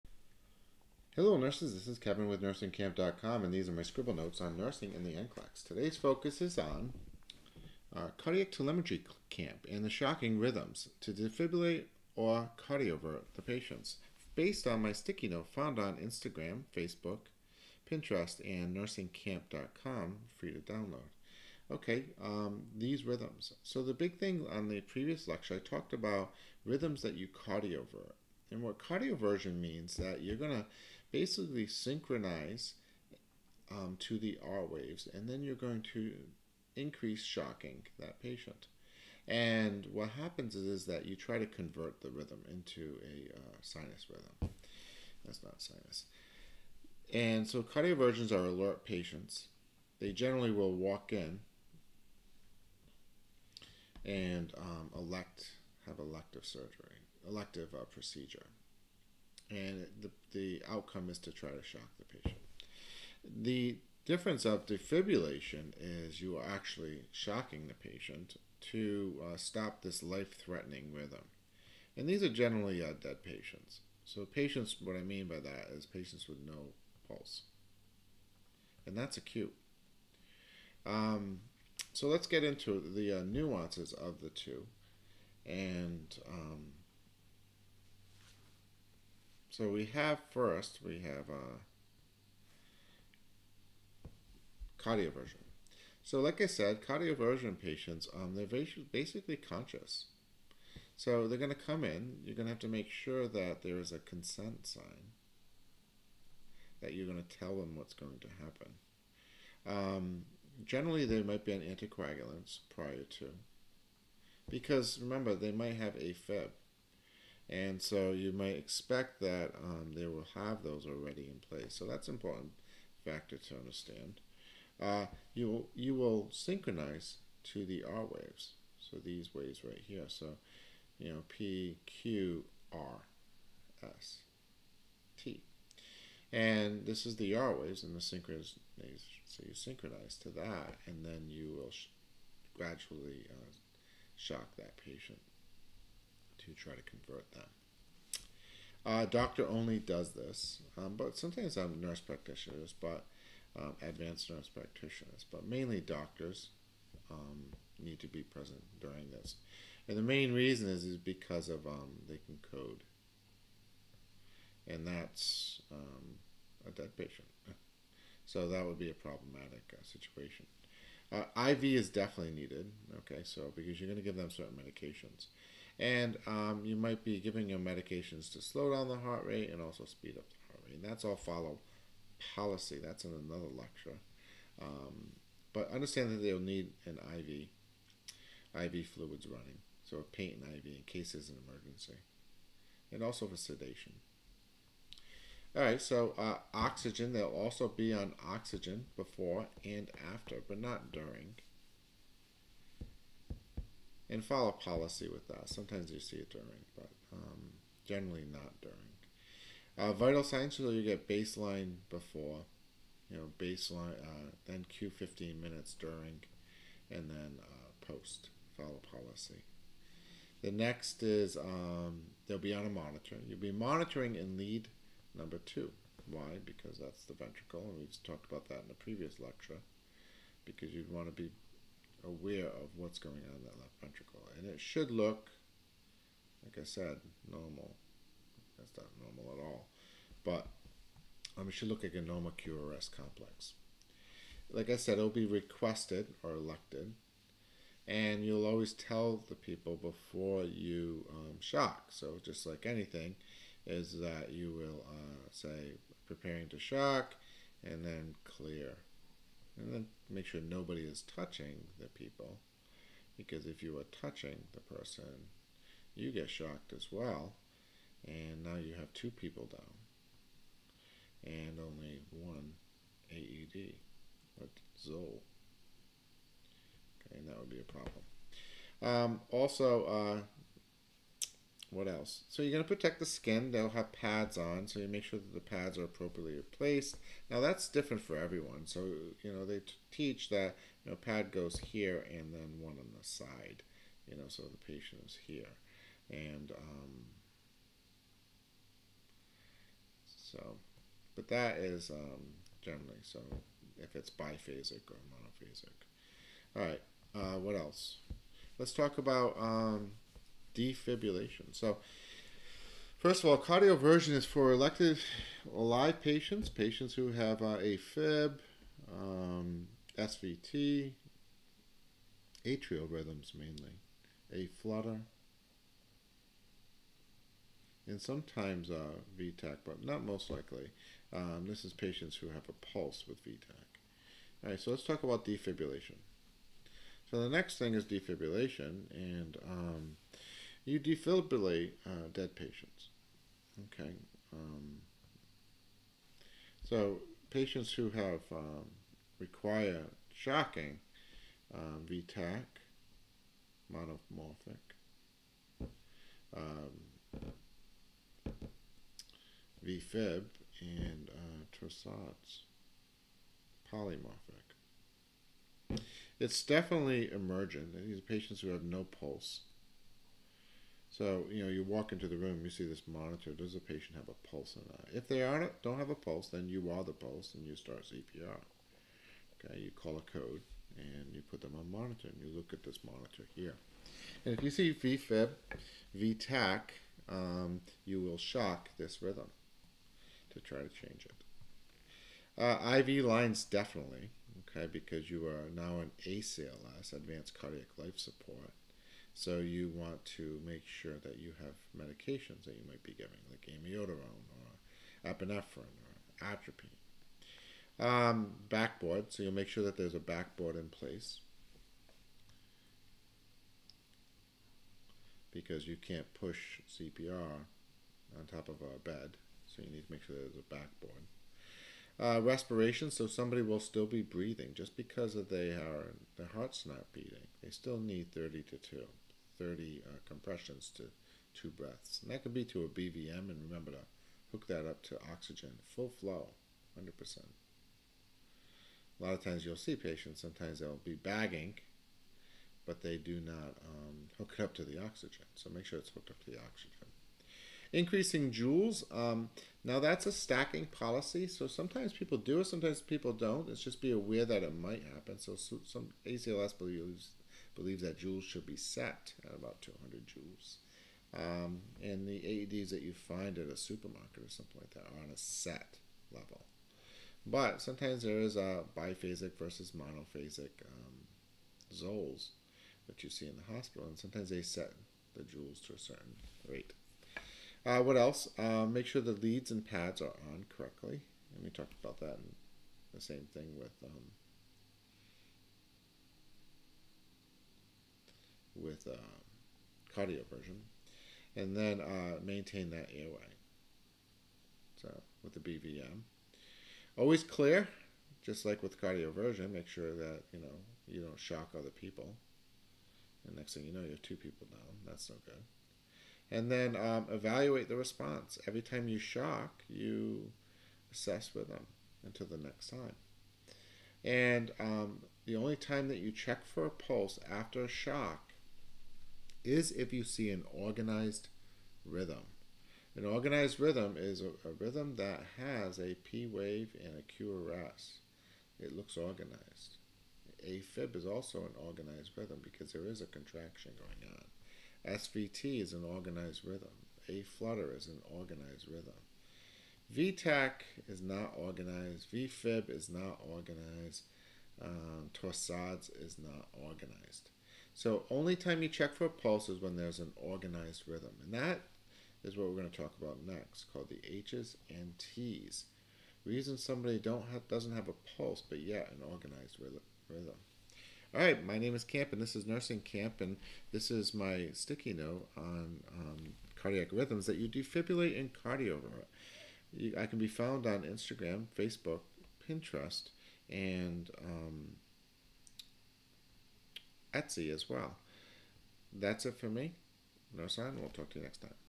What are Ventricular Rhythms Defib Cardioversion Nursing KAMP NCLEX 2019
In this video I cover my study sheet on ECG  Interpretation and nursing interventions like defibrillation and cardioversions from a study sheet that covers and EKG and ECG Ventricular Rhythms from my cardiac unit lectures found on nursingkamp.com  for NCLEX and Nursing

This studysheet is included in your membership on nursingkamp.com!Nursingkamp.com Telemetry Lecture 47 Ventricular Rhythms Defib Cardiovert Nursing KAMP Free Study Sheets at Pinterest

 @Nursing KAMP